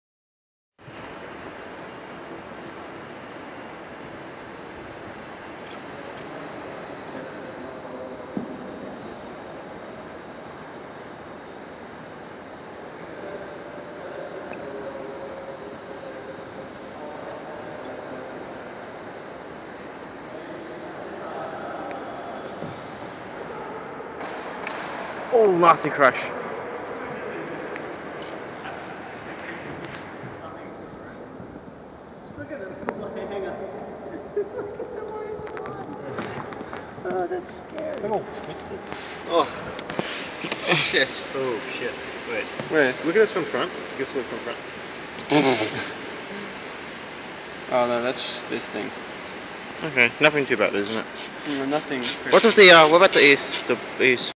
DBF 2011 - Testing - Crash (Wallplant) #3 (Closeup)
The 3rd crash into a wall during testing...